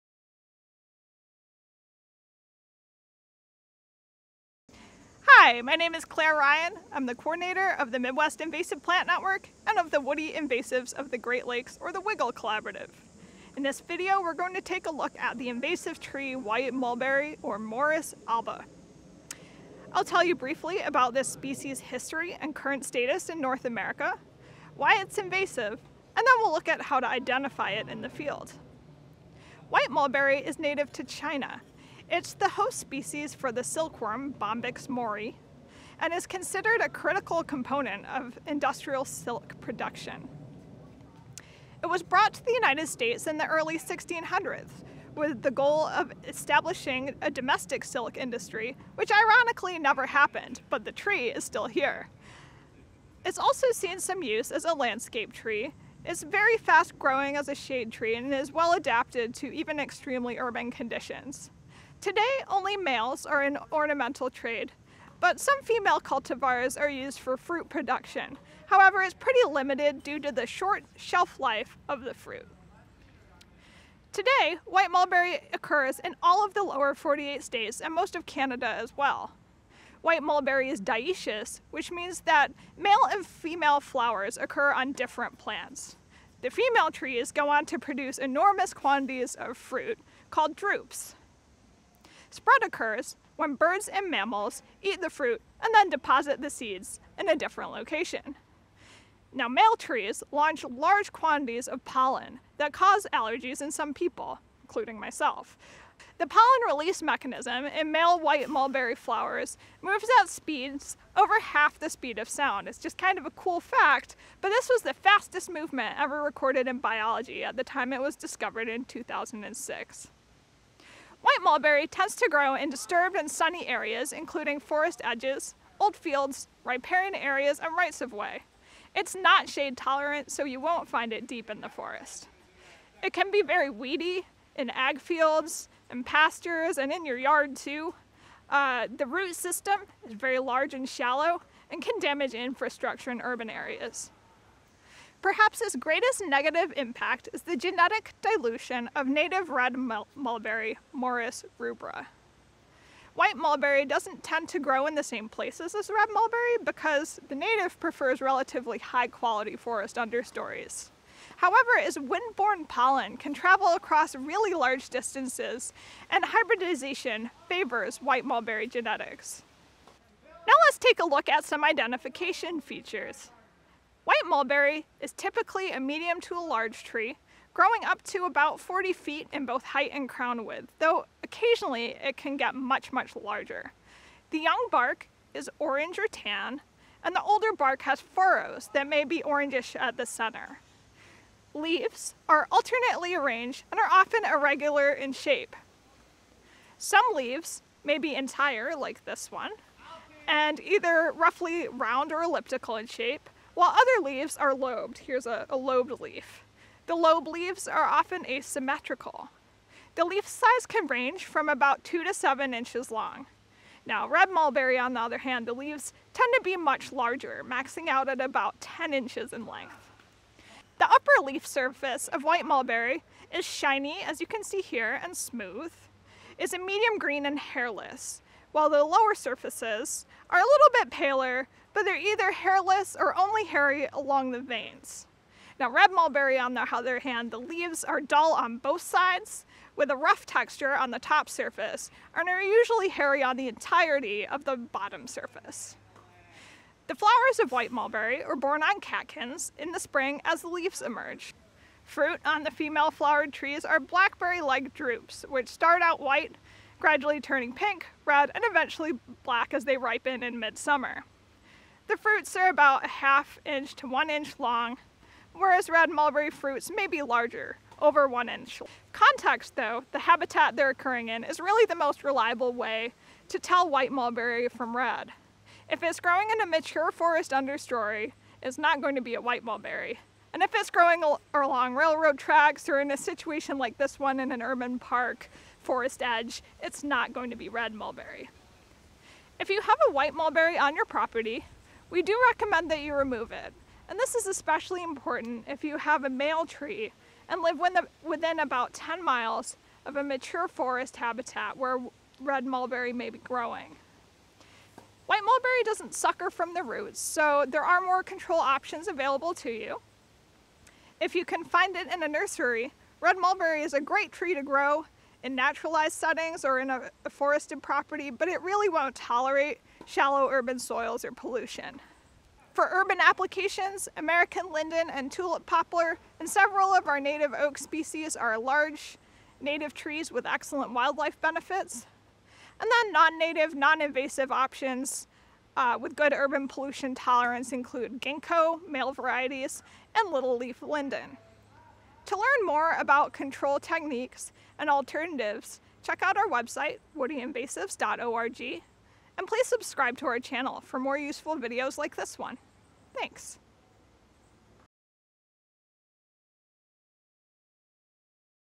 Identify invasive trees: White mulberry
It can be difficult to identify trees with variable leaves like white mulberry (Morus alba)! Learn all the tips and tricks here, and especially learn the differences between white mulberry and closely related but relatively rare native species red mulberry, Morus rubra. We also discuss why the spread of white mulberry as an invasive is a threat to red mulberry in particular.